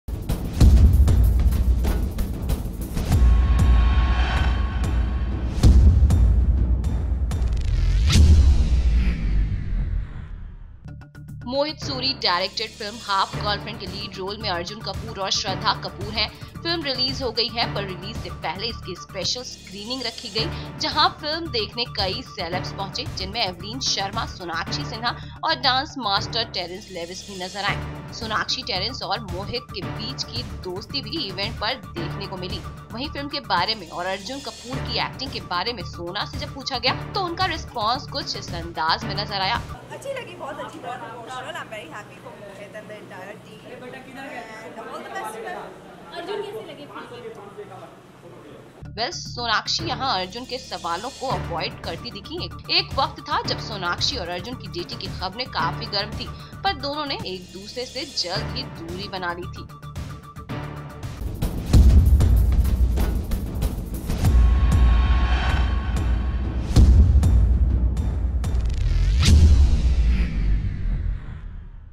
"Half Girlfriend" Special Screening | Sonakshi Sinha | Mohit Suri | Arjun Kapoor
मोहित सूरी डायरेक्टेड फिल्म  हाफगर्ल फ्रेंड के लीड रोल में अर्जुन कपूर और श्रद्दा कपूर हैं। फिल्म रिलीज हो गई है पर रिलीज से पहले इसकी स्पेशल सक्रिनिंग रखी गई जहां फिल्म देखने कई सेलेब्स पहुंचे जिनमें ऐवलीन शर्मा, सोनाक्षी सिन्हा और डांस मास्टर टेरेंस लेविस भी नज़र आये। सोनाक्षी टेरेंस और मोहित के बीचकी दोस्ती भी इवेंट पर देखने को मीली वहीं फिल्म के बारे में और अर्जुन कपूर की एक्टिंगके बारे में सोना से पुछा गया तो उनका रिस्पॉस कुछ इस अंदाज में नज़र आया .सौनाक्षी यहां अर्जुन के सवालको अव्वाइट करती दिखी एक वक्त था जब सौनाक्षी और अर्जुन की डेटिंग की खबरें काफी गर्म थी पर दोनो ने एक दूसरे से जल्द ही दूरी बना ली थी।

बॉलीवुड / मूवी / फिल्म / गाने / फिल्में